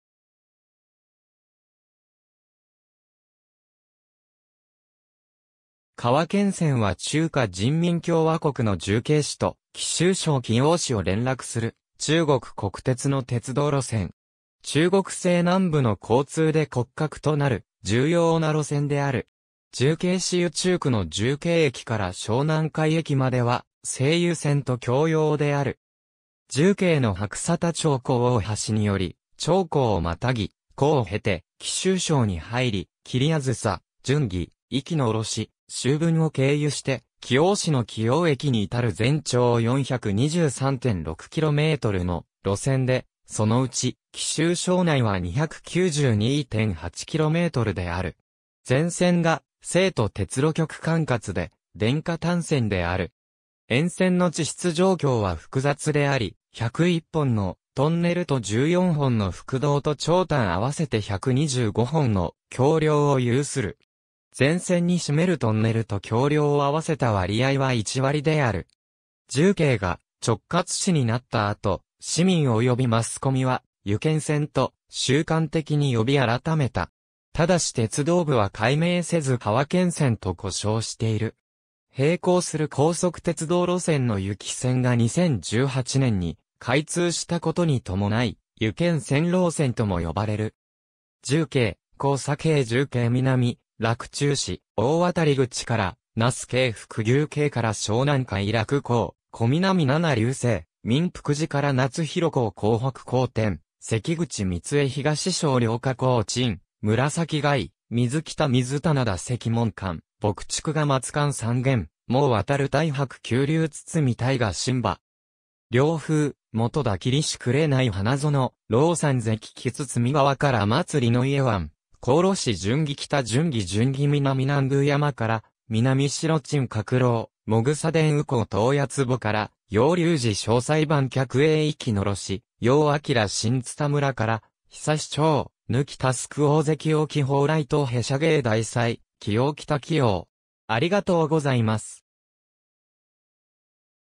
川黔線
川黔線（せんけん-せん）は中華人民共和国の重慶市と貴州省貴陽市を連絡する中国国鉄の鉄道路線。
中国西南部の交通で骨格となる重要な路線である。
重慶市渝中区の重慶駅から小南海駅までは成渝線と共用である。
重慶の白沙沱長江大橋により長江を跨ぎ、綦江を経て貴州省に入り、桐梓、遵義、息烽、修文を経由して貴陽市の貴陽駅に至る全長423.6kmの路線で、そのうち貴州省内は292.8kmである。
全線が成都鉄路局管轄で、電化単線である。
沿線の地質情況は複雑であり、101本のトンネル（総延長29352m）と14本の覆道（総延長5642m）と長短併せて125本の橋梁（総延長10.1km）を有する。
全線に占めるトンネルと橋梁を併せた割合は一割である。
重慶が直轄市になった後、市民及びマスコミは渝黔線と習慣的に呼び改めた。
但し鉄道部は改名せず川黔線と呼称している。
平行する高速鉄道路線の渝貴線が2018年に開通したことに伴い、渝黔線老線（旧線）とも呼ばれる。